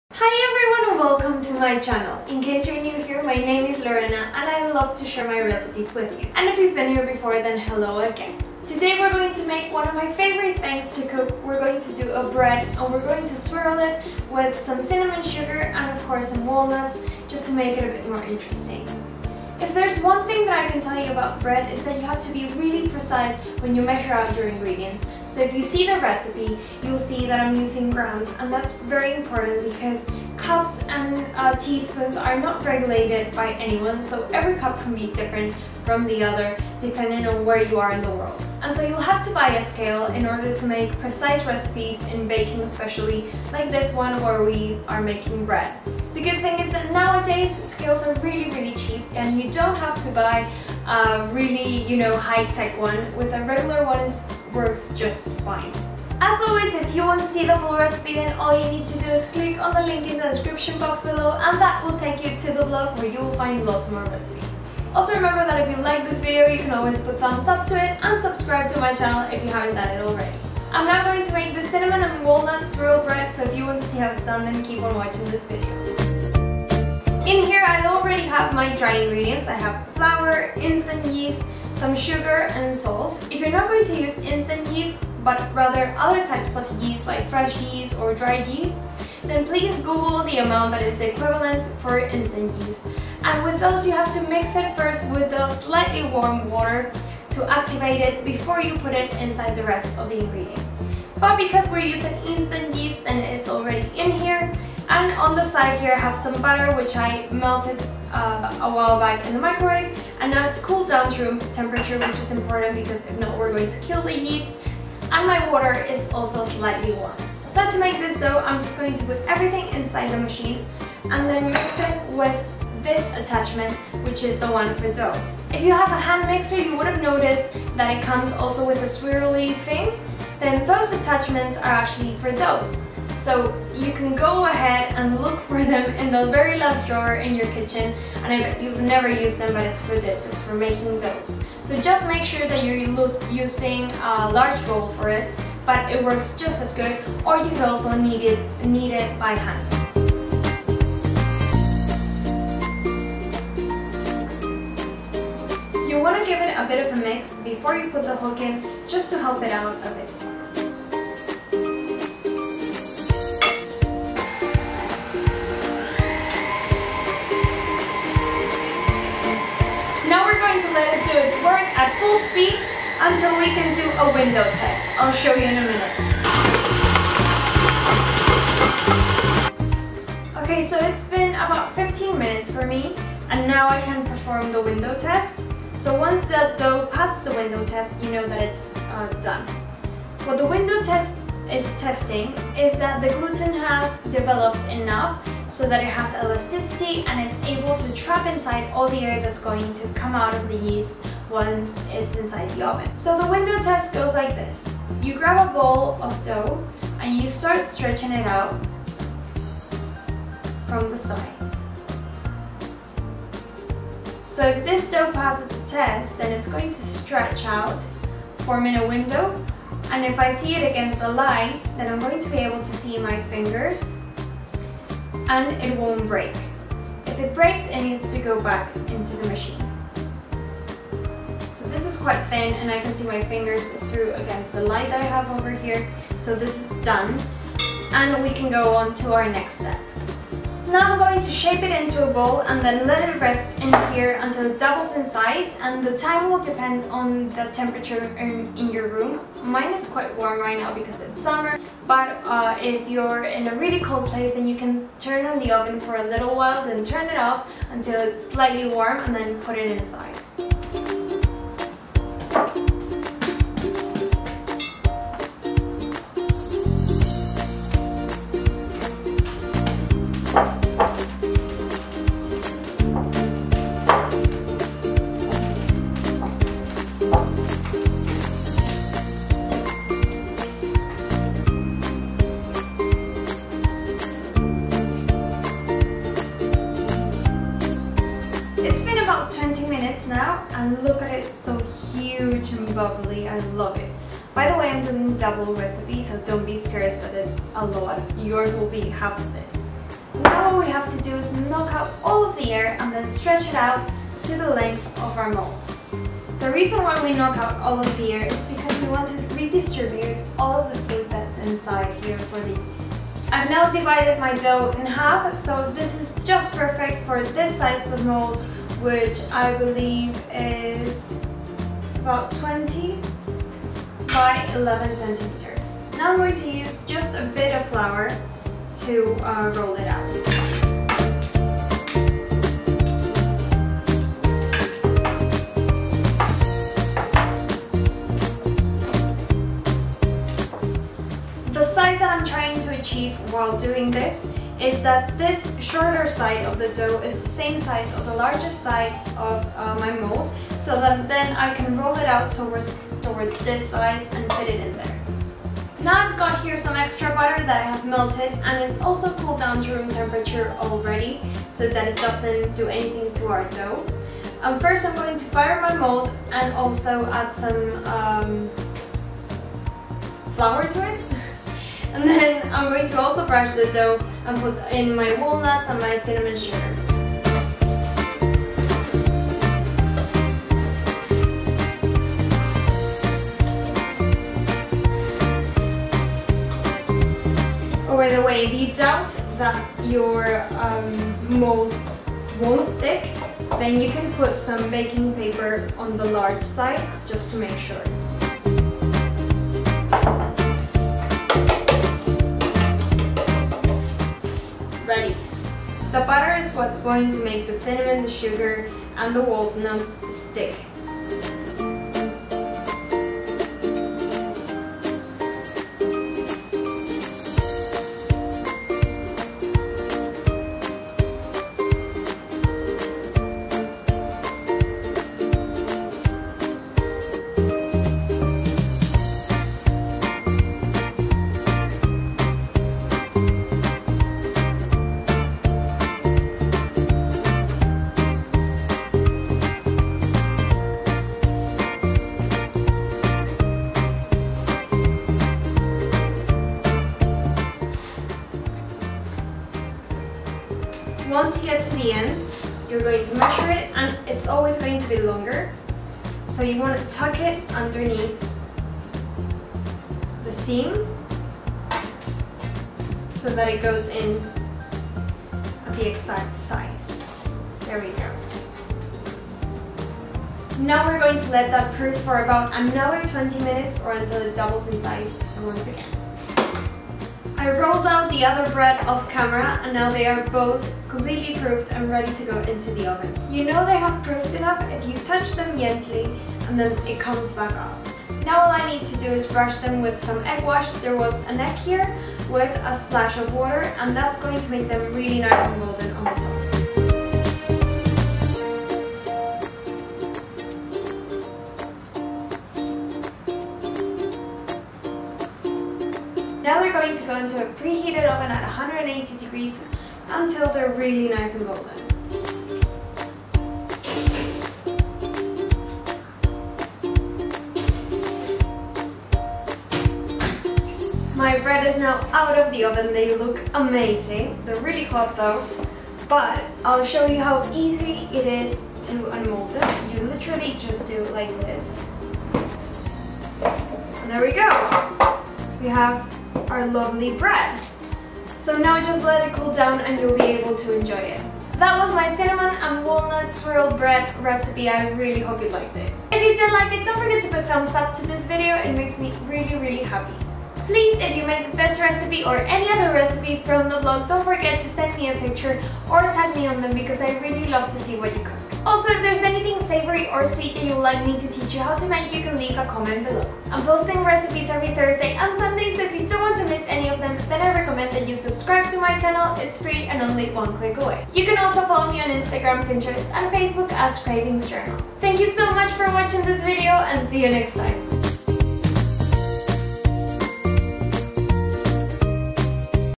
Cinnamon and Walnut Swirl Bread | Cravings Journal
Is there anything nicer than the smell of fresh bread? Now imagine if mixed with that there's the incredible smell of cinnamon... Just so perfect. This bread dough you can also use to make a regular loaf of bread of you can even braid it and do pull apart bread. It's the perfect recipe to vary.